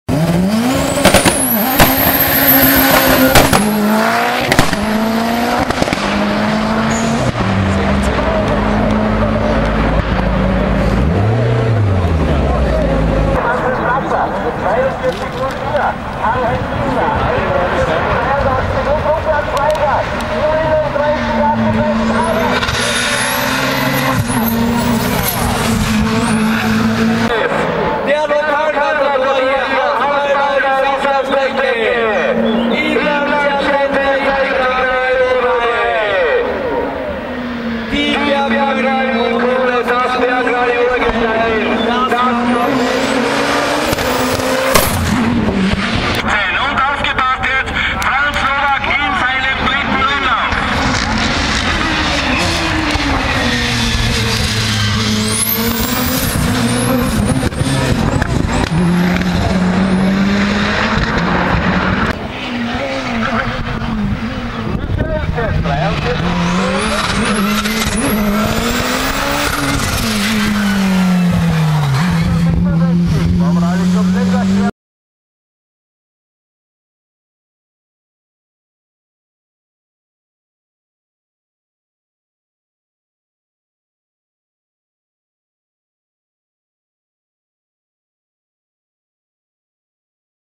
730Hp Lancia Delta "Yellow Bull" // Crazy Turbo Sound - Franz Nowak´s Beast Last Race
Franz Nowak´s Yellow Bull is one of the most outrageous Monsters ever to step in Hillclimb Racing with its 700+Hp 2.0 Turbo engine and only 1050Kg of weight.

Unfortunately the last races where this machine were used competitively were on 2013, and since then it as been put up for sale last year. Well, this is the chance for a very lucky person to by a piece of Hillclimb History. And one that was once did a 0-100Km/h sprint in 1,5 seconds...